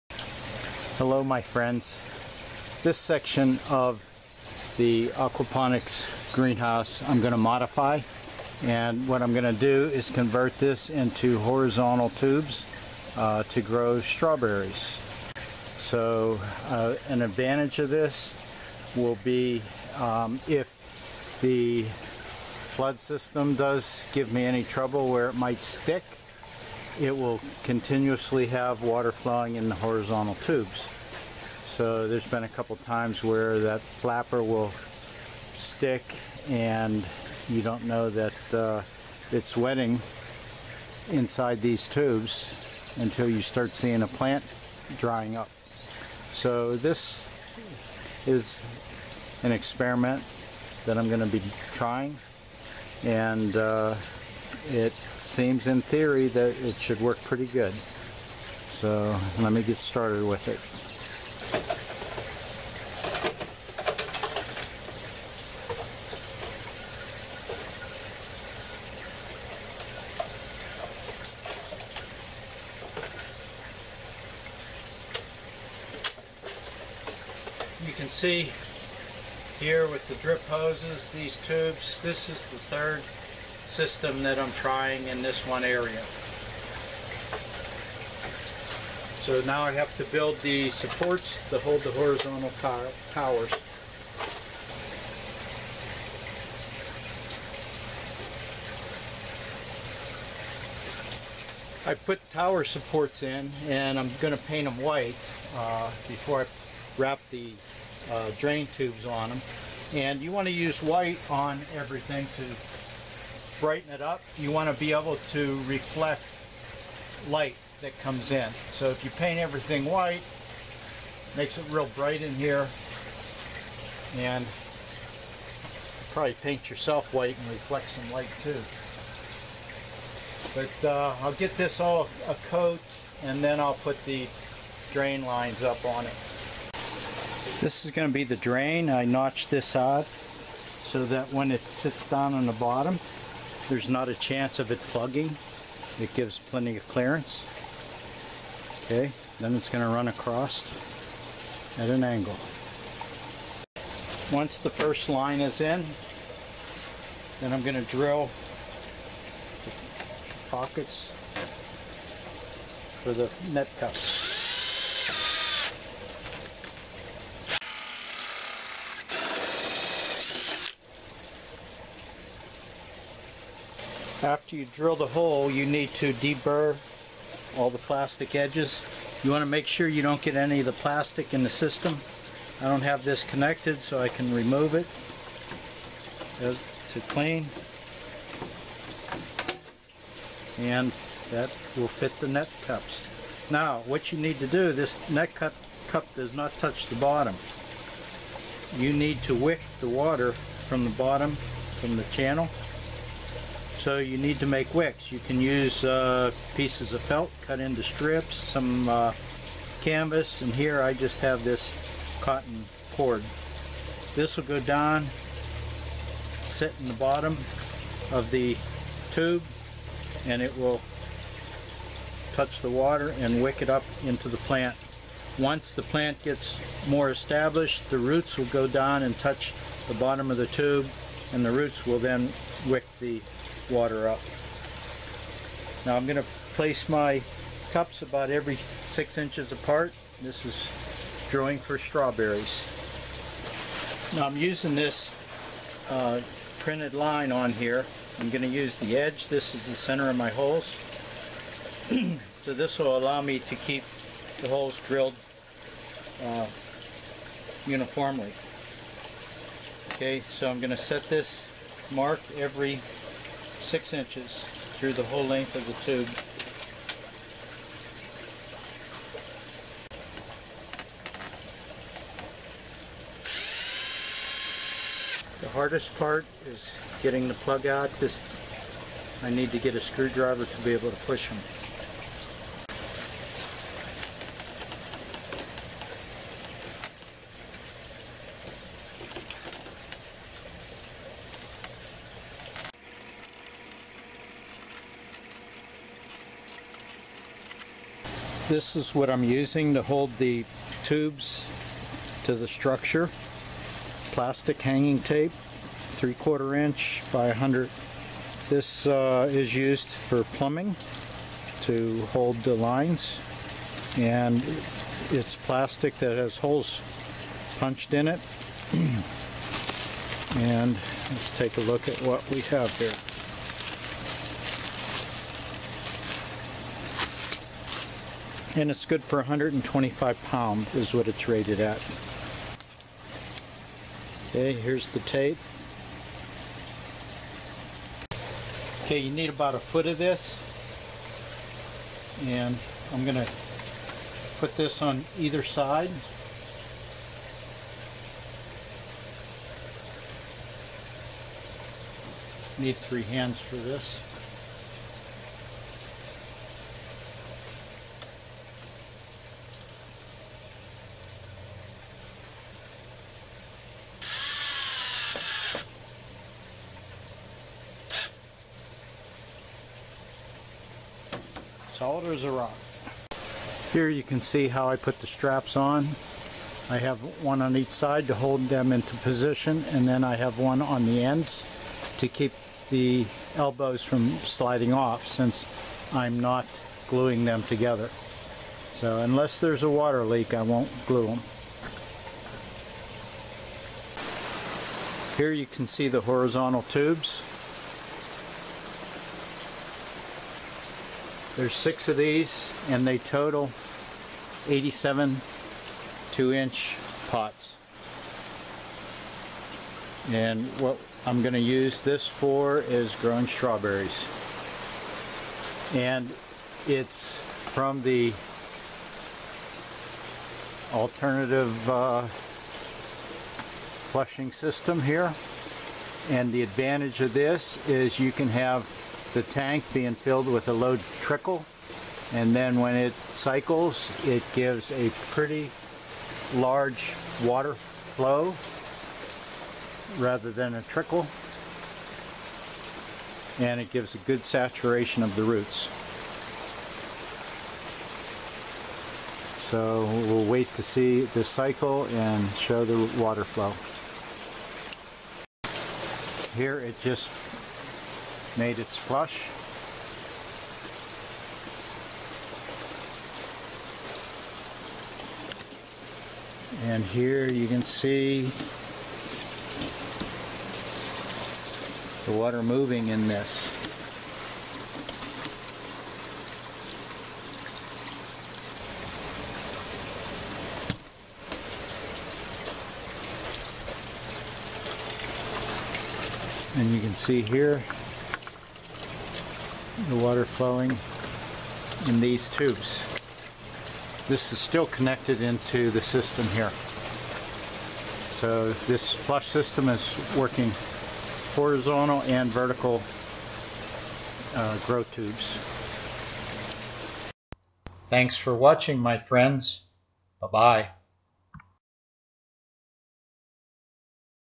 Build Horizontal Grow Tubes in Aquaponics System
Building Horizontal Grow Tubes that will use the large volume flush watering system. This is a modification to the Aquaponics rebuild of my system.